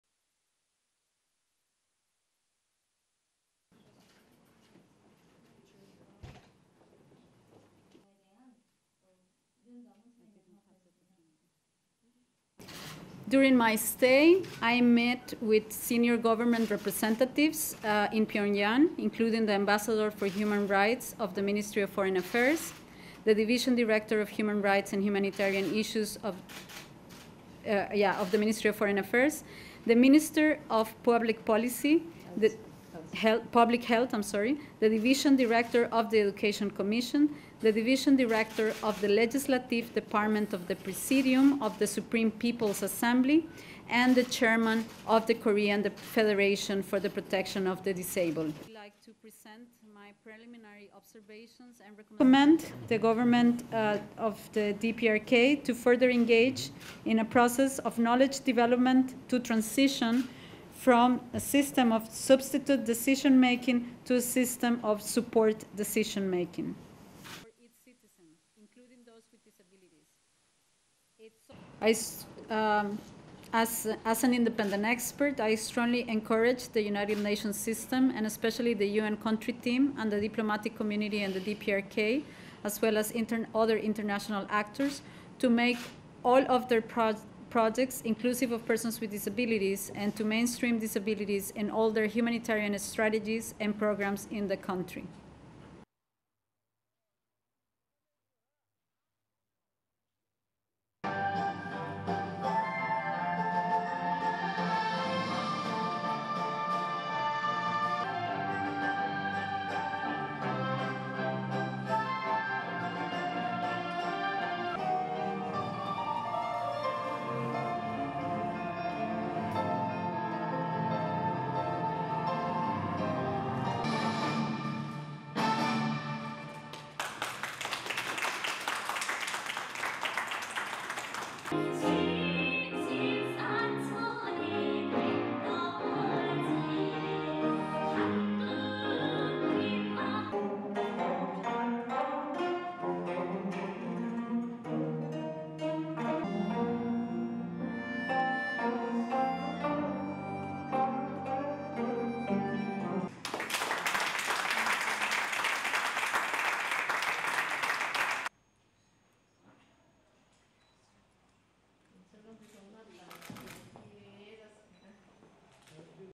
UN official in NKorea reports on disabled rights
(8 May 2017) A top UN official visiting North Korea called on Monday for more attention to be given to disabled people in the socialist state.
UN Special Rapporteur on the rights of persons with disabilities Catalina Devandas Aguilar talked to a small group of foreign and North Korean media at a news conference on the last full day of her 8-day trip to Pyongyang.
Aguilar is the first UN official designated by the international body's Human Rights Council to get into North Korea.
Her job is not the same as her UN colleague Tomas Ojea Quintana, who has the title of UN Special Rapporteur on the situation of human rights in the DPRK.
Neither he nor his predecessors have been to North Korea.
In recent years, Pyongyang authorities have tried to promote taking care of people with disabilities, as international criticism of human rights in North Korea has grown.